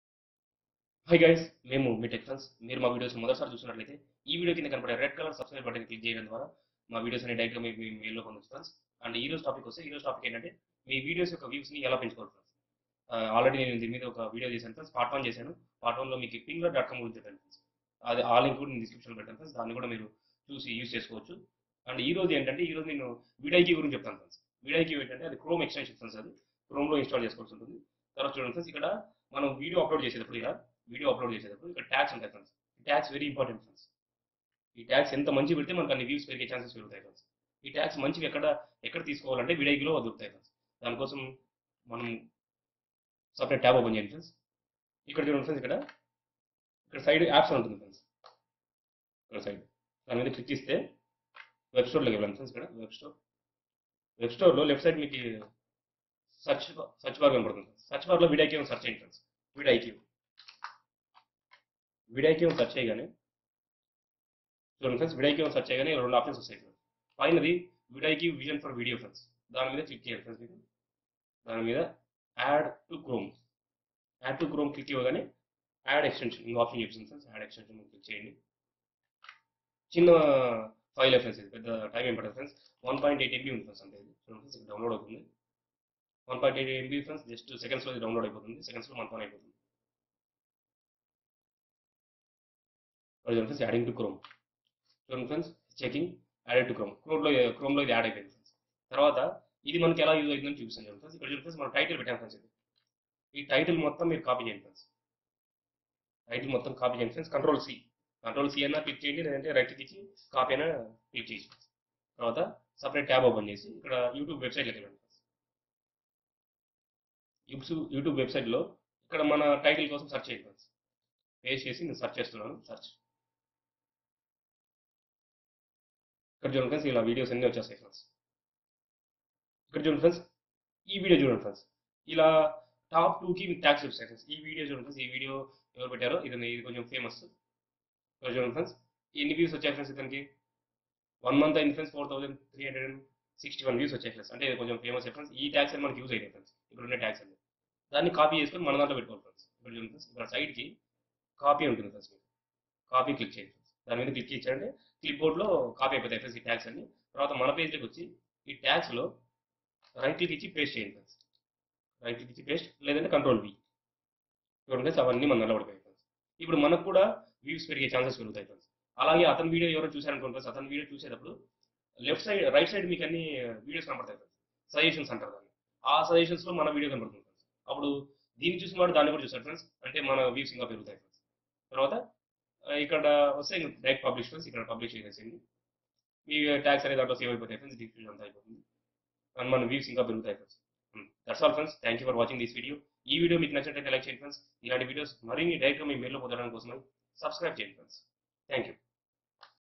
How To Get More Views And Subscribers On YouTube || Telugu Tech Buzz
Open Upstox FREE Account from the Links below and starting trading and make money online and also get a free demat account

. telugutechbuzz Hey Guys this the part 2 of How To Get More Views And Subscribers On YouTube In This Cool Video we will be teaching you How to Use Most Popular and most Effective trusted Chrome Extension called VidiQ..

VidiQ is the best tool for Passionate You tubers, VidiQ has many features like, Bulk Seo, Tag Suggestion , Advanced Video Statistics and many more Powerful Features...

Vid iQ is free to Use and I Highly Recommend this Tool